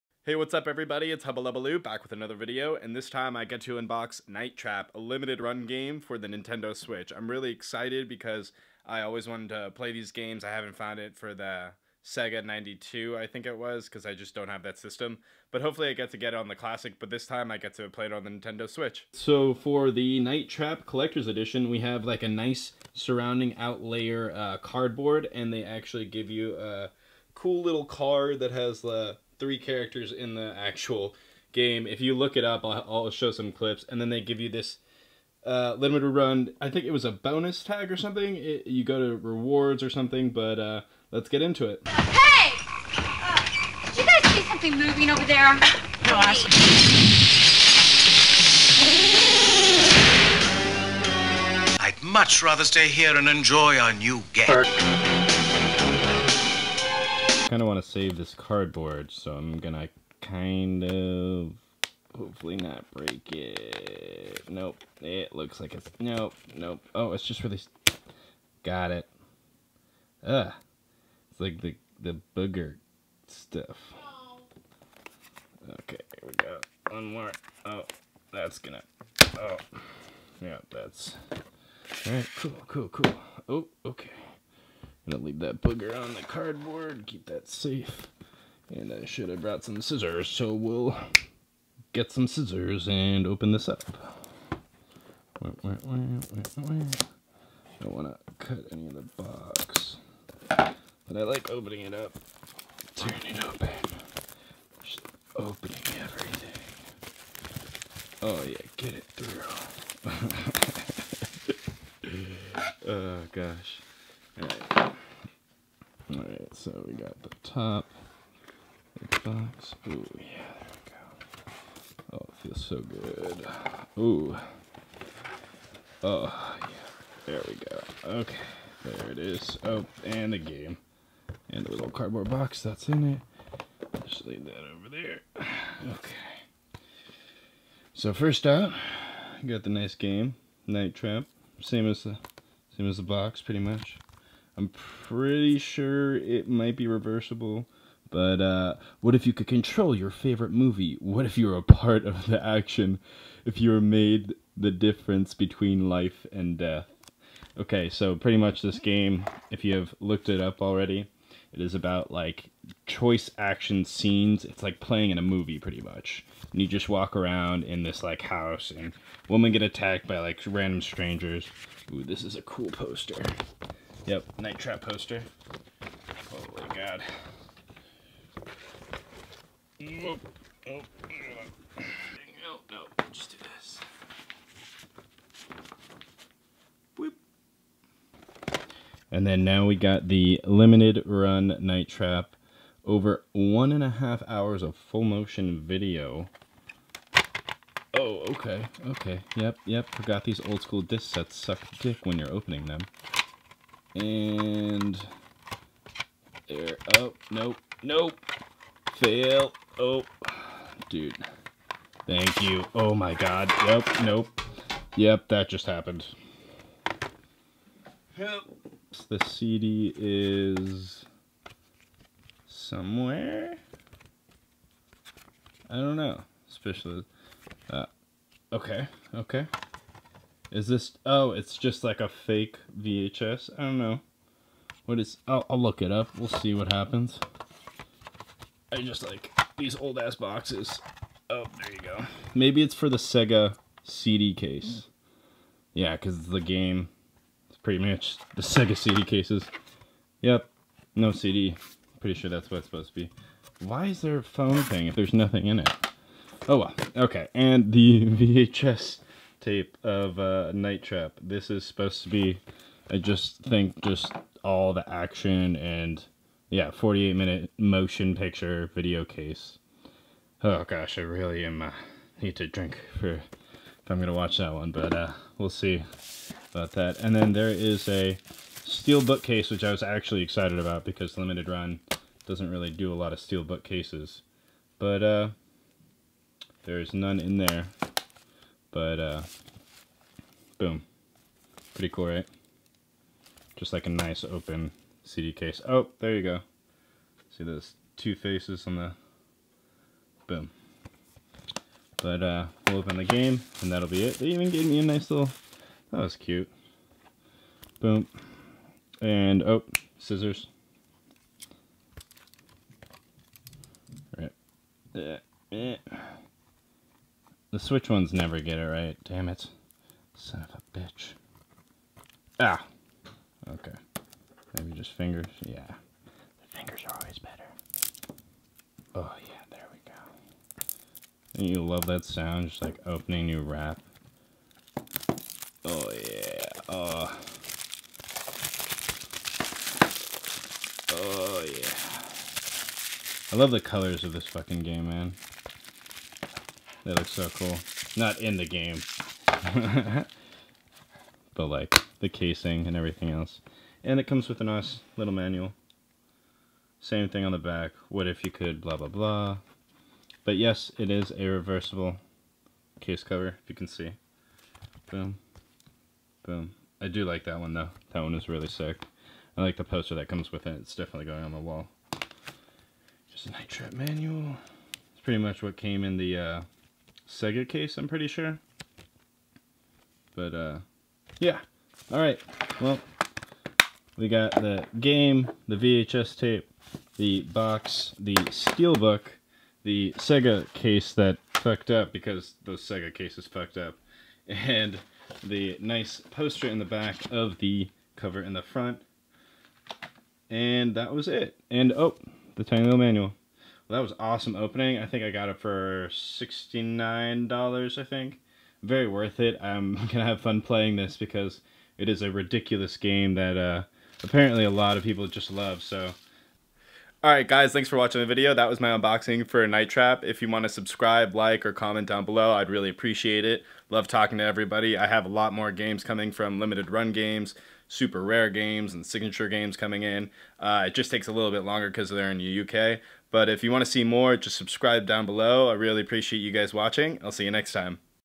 NIGHT TRAP! Collector's Edition - #8 Limited Run Games Nintendo Switch Unboxing
Hey guys unboxing another awesome old school game back from the Sega 92! Made by Limited Run Games I can't wait to post and play this game!